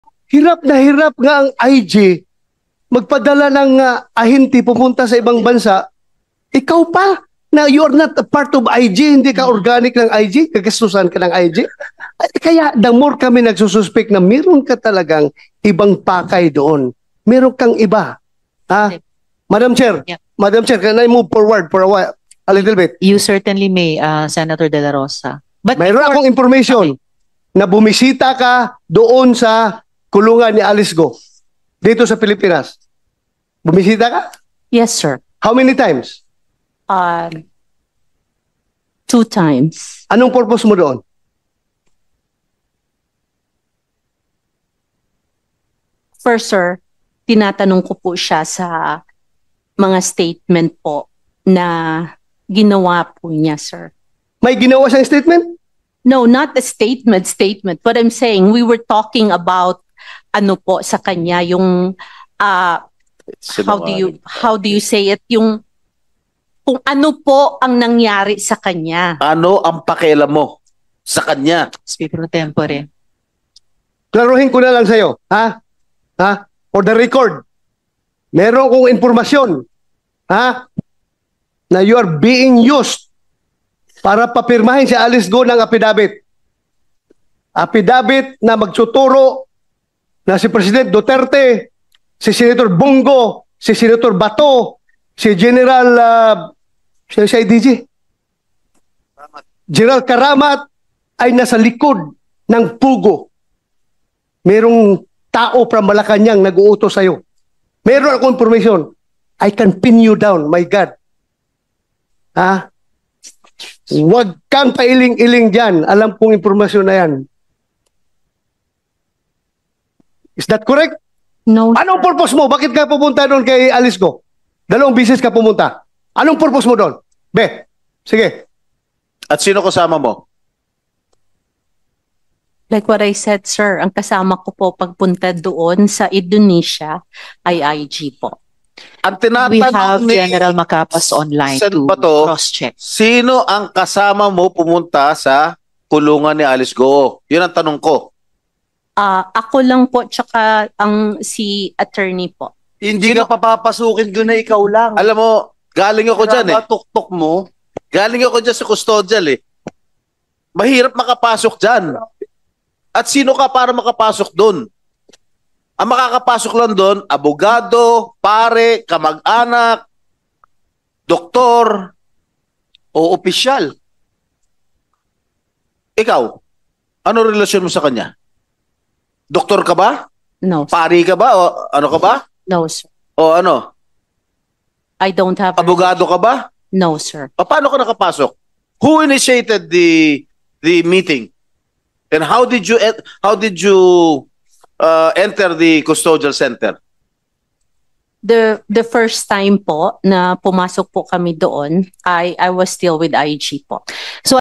MARRY ANN MASLOG INUTOSAN DAW UMANO NG TAGA MALACAÑANG.
MARRY ANN MASLOG INUTOSAN DAW UMANO NG TAGA MALACAÑANG PARA SABIHIN KAY ALICE GUO NA IDAWIT SINA SEN. DELA ROSA, SEN. BONG GO AT FPRRD SA POGO.